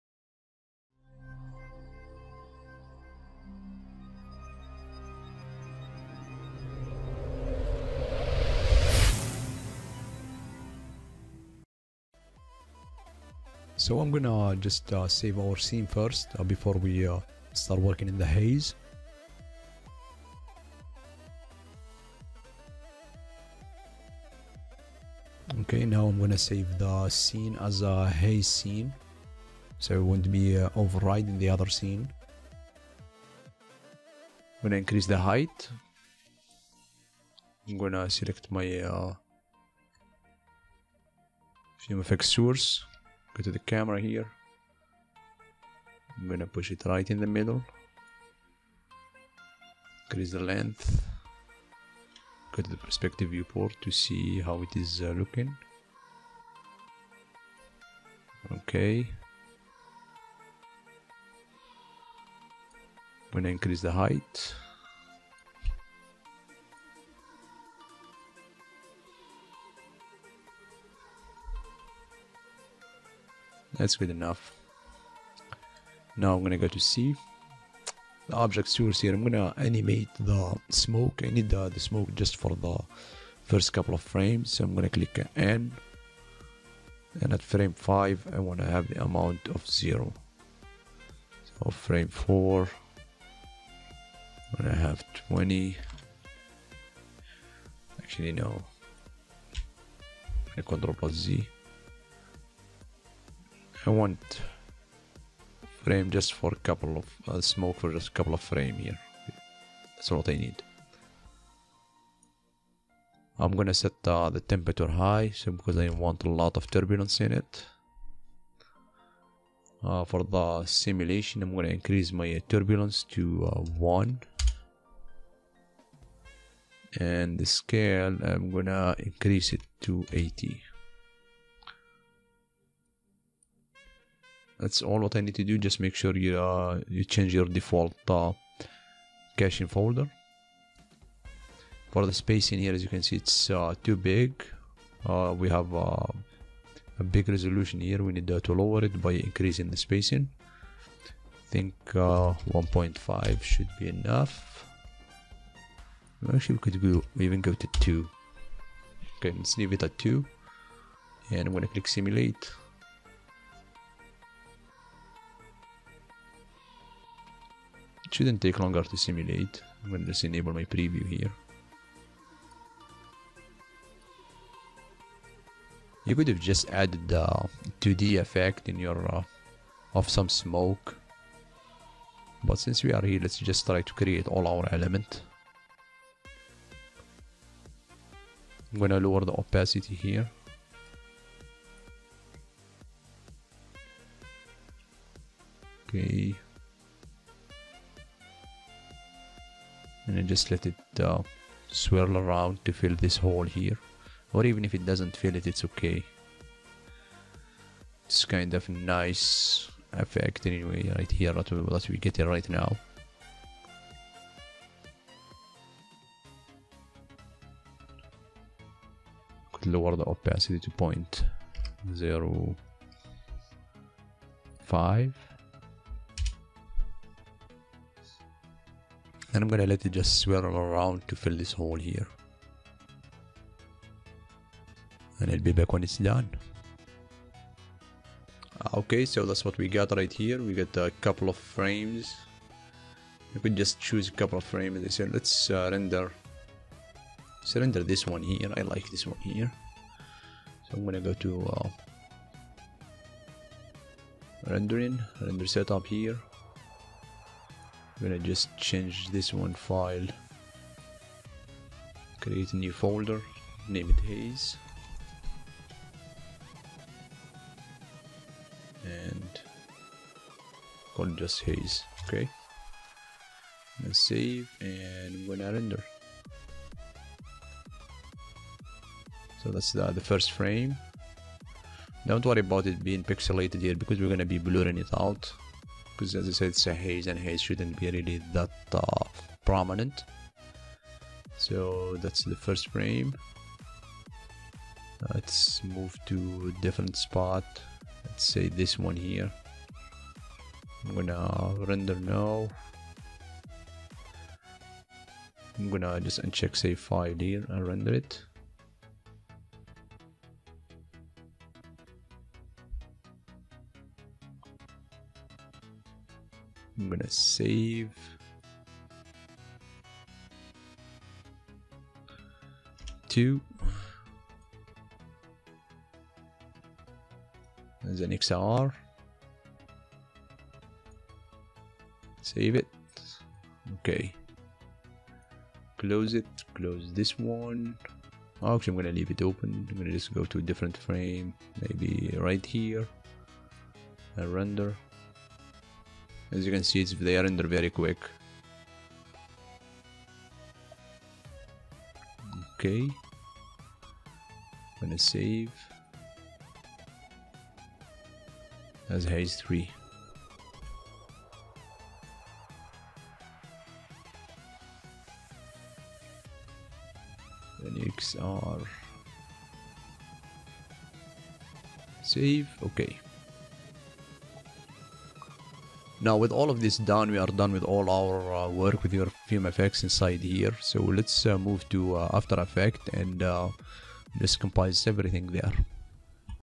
Create amazing cloud scene part 3 : the haze
we continue our pyro month. in this tutorial we will create a beautiful puffy cloud scene , we will use particle flow , fumefx and after effect to achieve the result we want so sit back enjoy and learn.